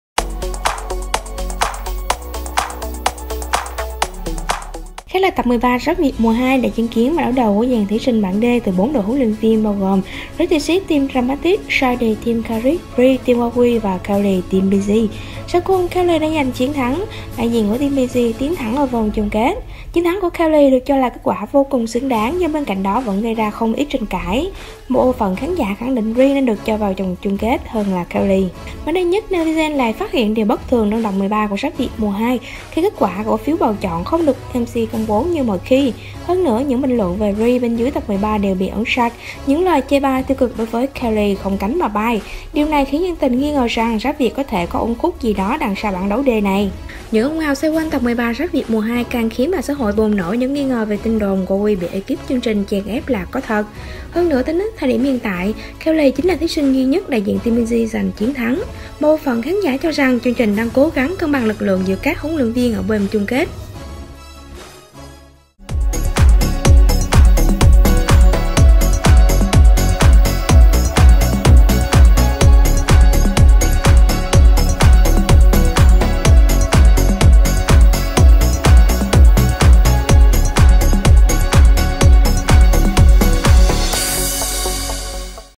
Uẩn khúc sau chiến thắng của Kellie và Free bị xoá sạch bình luận? Chuyện cơ cấu là thật?
Chiến thắng của Kellie và Free bị xoá sạch bình luận? Chuyện cơ cấu là thật tại Rap Việt Mùa 2 để vào Chung kết?